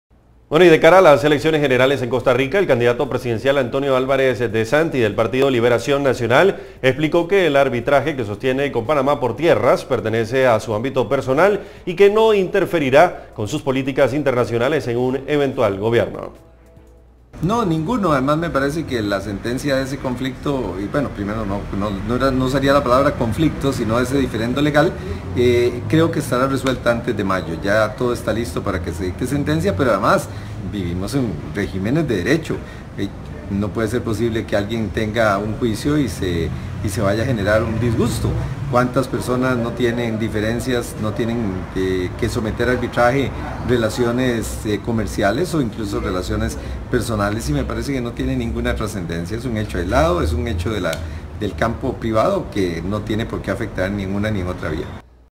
Álvarez: Arbitraje por tierras con Panamá es personal y no interferirá en relaciones con Costa Rica
De cara a las elecciones generales en Costa Rica, el candidato presidencial Partido Liberación Nacional,  Antonio Álvarez Desanti explicó que el arbitraje por tierras que sostiene con Panamá pertenece a su ámbito personal y que no interferirá con sus políticas internacionales en un eventual gobierno suyo.

Redacción CB24